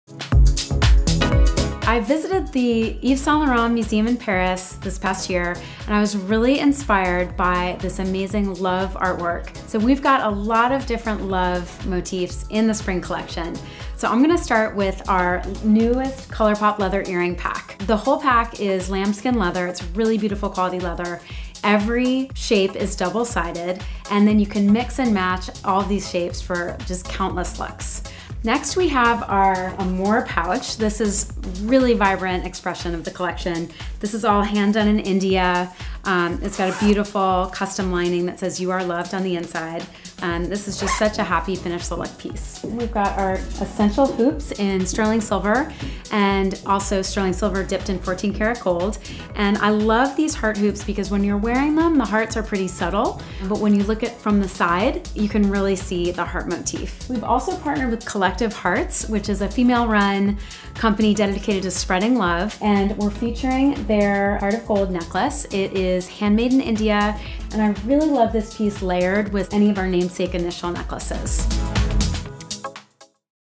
It's All About LOVE | Spring 2019
The Stella & Dot Spring collection is all about colorful, optimistic artwork that we saw from a recent trip to Paris. We've put it all together in complete looks you'll love.